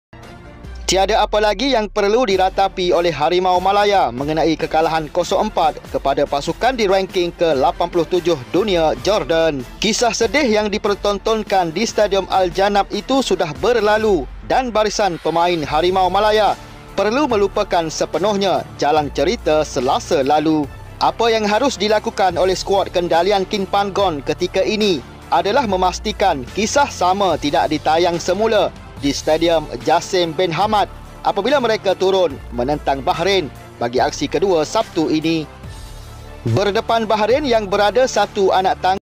"MOVE ON" MALAYSIA LAWAN BAHRAIN LUPAKAN KISAH SEDIH
piala Asia 2023 Malaysia vs Bahrain

Harimau Malayakim pan-gonlupakan kekalahan kepada jordankim pan gon